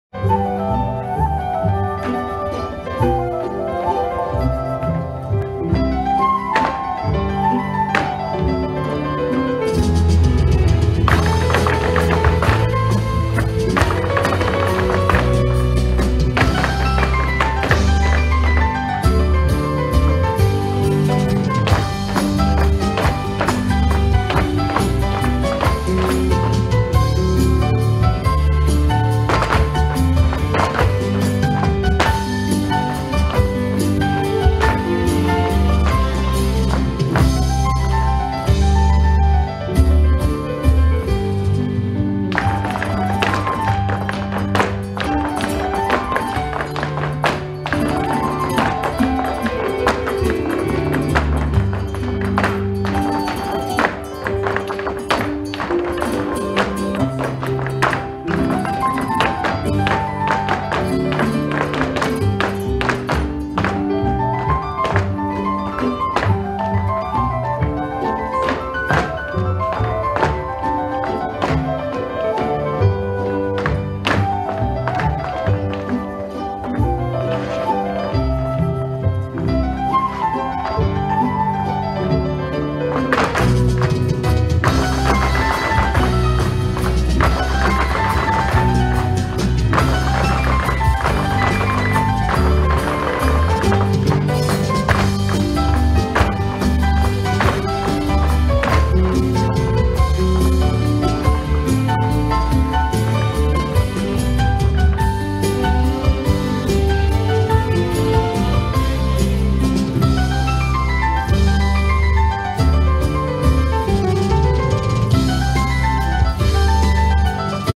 Bonegio Spanish Dance Company - Salinas 2021
A contemporary Flamenco dance piece from our show, "Flamenco Al Aire Libre", at the Dome at NSA - November 2021.

Choreography: Julia Bonegio.
Dancers: Julia Bonegio, Charo Romano, Raquel Bonegio, Natalia Bonegio.